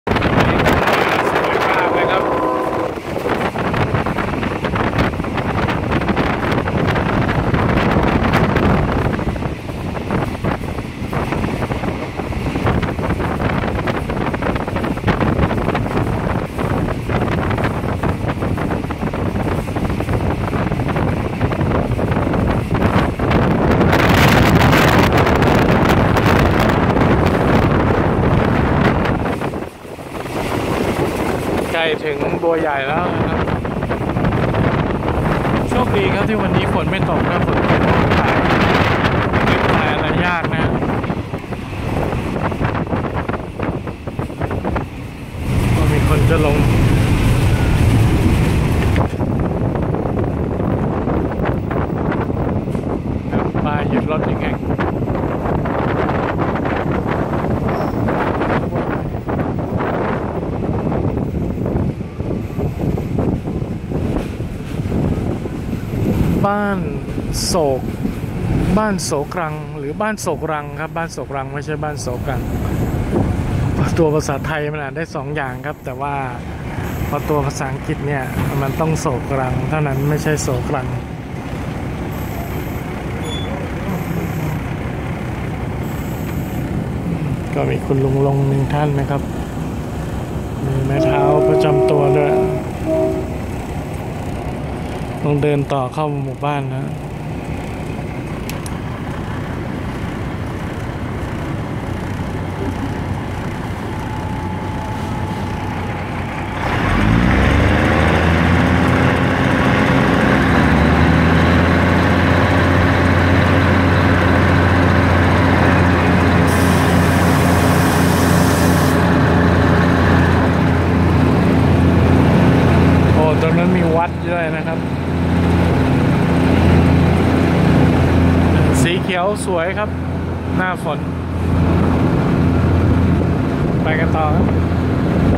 ป้ายหยุดรถไฟบ้านโสกรัง | หนองบัวสะอาด
ป้ายหยุดรถไฟบ้านโสกรัง  ตำบลหนองบัวสะอาด อำเภอบัวใหญ่ จังหวัดนครราชสีมา

นั่งมาากแก่งคอยไปบัวใหญ่ครับ ขบวน 349  เป็นเส้นทางแยกขึ้นสายอีสาน